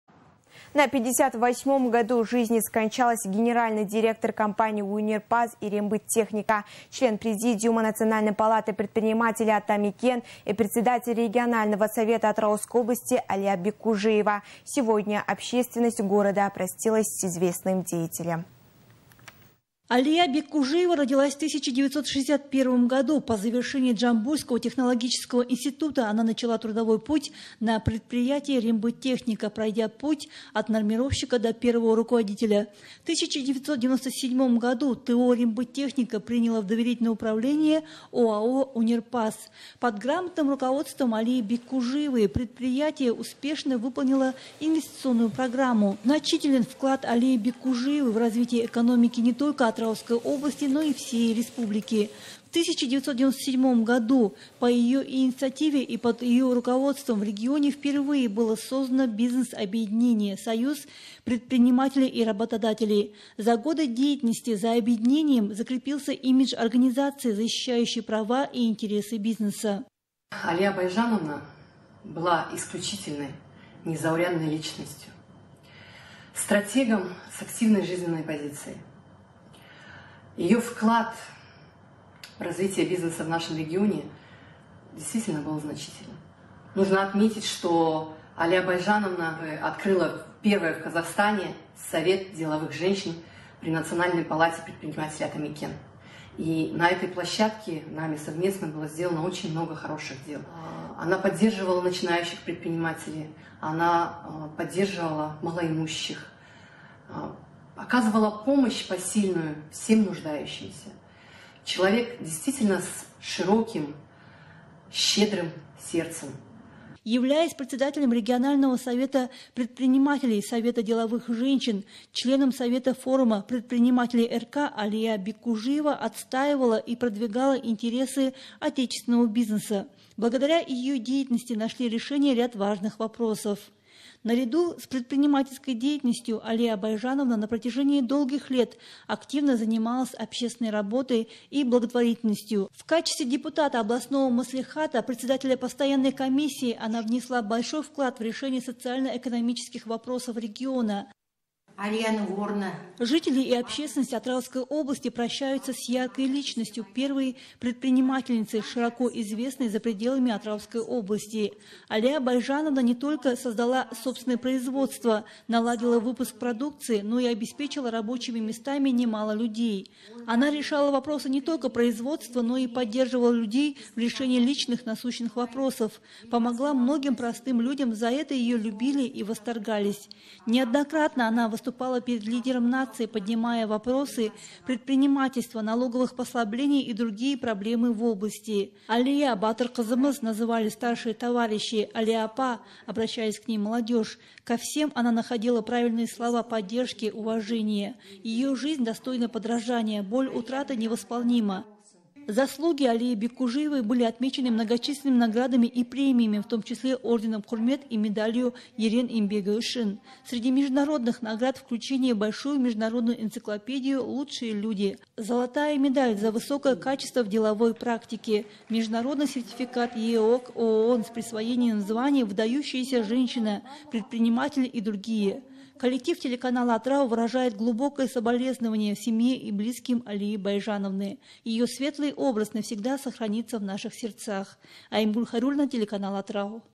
УШЛА ИЗ ЖИЗНИ ИЗВЕСТНЫЙ ОБЩЕСТВЕННЫЙ ДЕЯТЕЛЬ АЛИЯ БЕККУЖИЕВА
На 58-ом году жизни скончалась генеральный директор компаний «Онерпаз» и «Рембыттехника», член Президиума Национальной Палаты Предпринимателей «Атамекен» и председатель регионального совета Атырауской области Алия Беккужиева. Сегодня общественность города простилась с известным деятелем